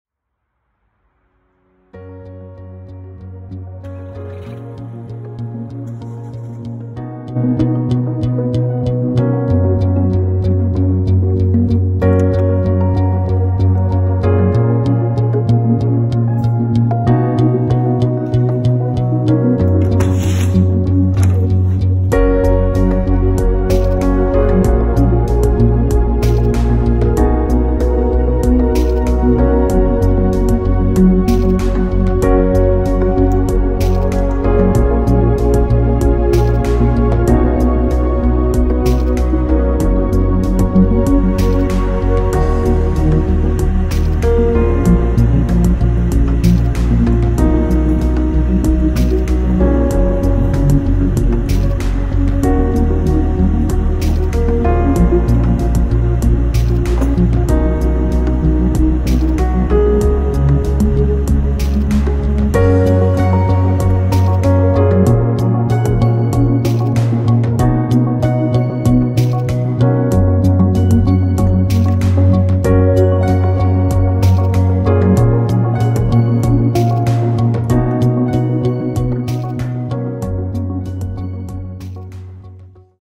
Morning coffee at my signwriting studio
Chilly start to my day, sun is trying to get through the early morning mist - looks gorgeous.

Making my coffee has got to be the first thing I do when I get into my studio.